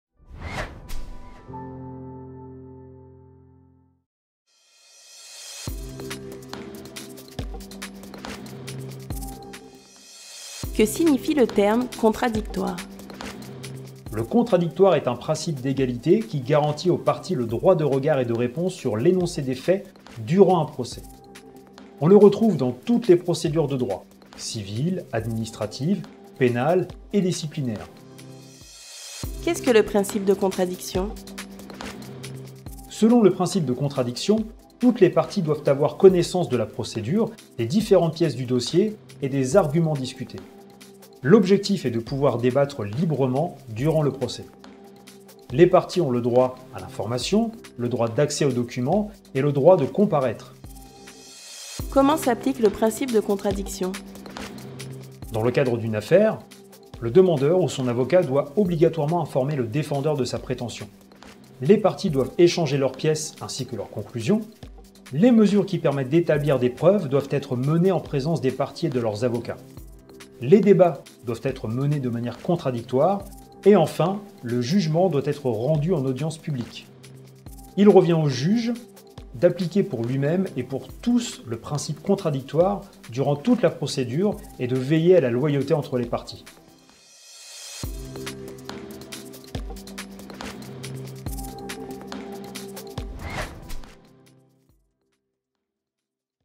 Le contradictoire, c'est quoi ? (définition, aide, lexique, tuto, explication)
Voici le sommaire de la video : Que signifie le terme “contradictoire” ?

Qu’est-ce que le principe de contradiction ?

Comment s’applique le principe de contradiction ?

Sujet : Julien Dupé
Rédaction : Chloë Tadros
Présentation : Mikael Buxton
Voix off : Chloée Bernos
Design : Gaëtan Lebaron
Réalisation : Florent Lahousse
Composition sonore : Grégoire Levillain